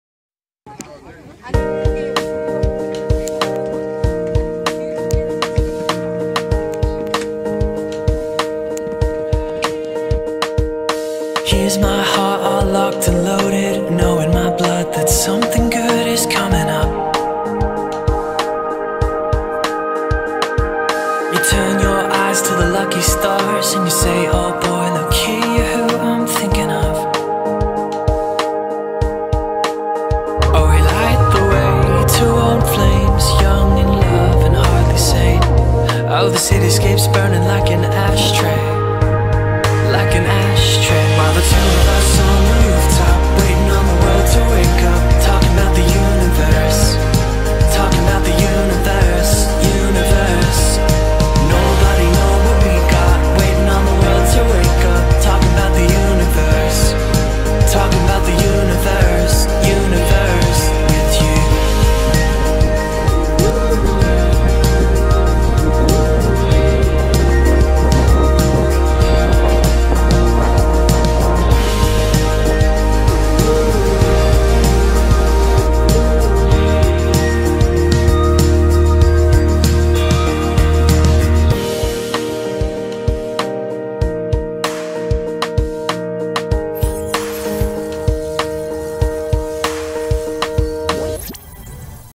Road Trip to Ladakh and Jeju
Leh to Nubra Valley to Pangong Tso
with some of Jeju Trip scenes and festival especially Seongsan Ilchulbong Sunrise